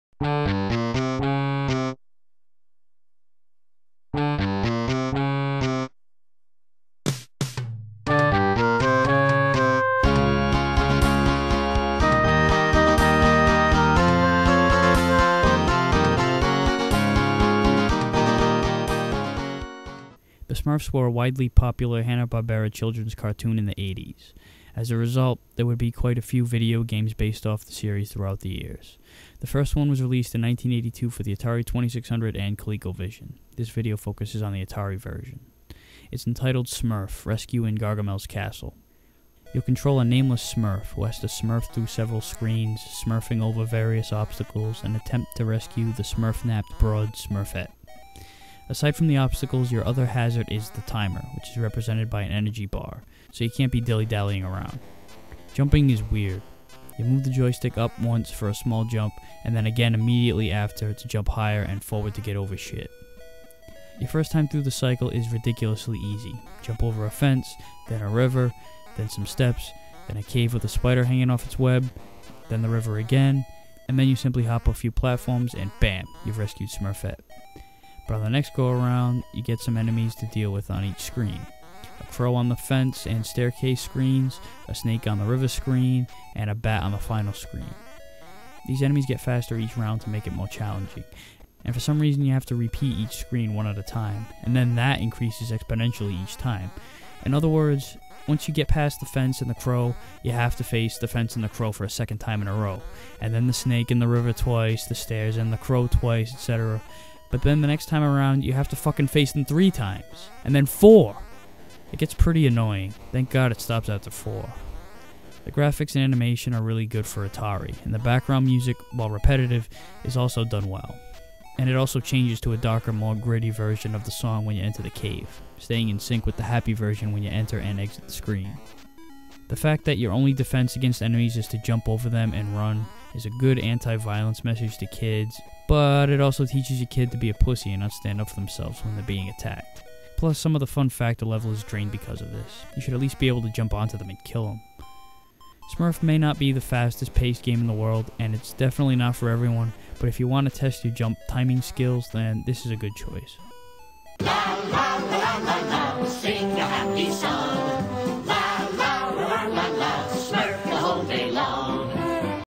Smurf Atari 2600 Review
My review of "Smurf: Rescue In Gargamel's Castle" for the Atari 2600.